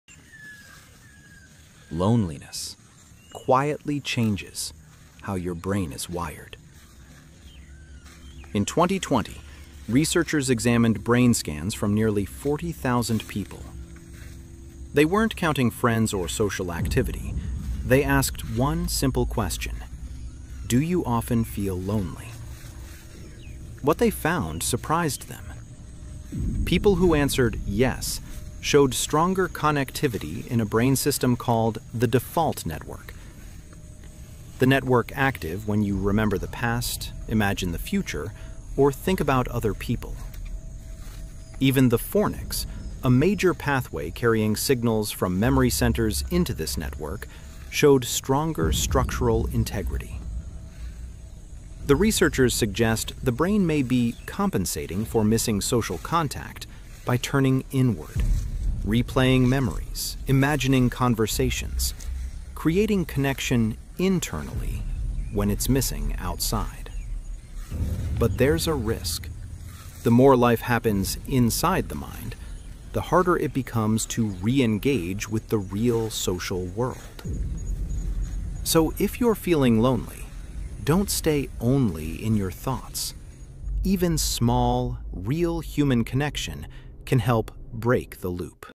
What a Study of 40,000 Brain Scans Reveals About Loneliness
Loneliness is usually described as an emotion.
But large-scale brain imaging suggests it’s also linked to measurable changes in how the brain works.

In this video, we look at findings from a 2020 neuroimaging study of nearly 40,000 adults, examining how perceived loneliness relates to changes in the brain’s default mode network  a system involved in memory, imagination, and thinking about other people.

Importantly, the study didn’t measure social activity or number of friends.
It asked one simple question: Do you often feel lonely?

The results help explain why loneliness can feel so consuming  and how the brain may adapt when social connection feels missing.

If you’re interested in neuroscience, psychology, and evidence-based explanations of human behavior, follow the page for more research-driven videos.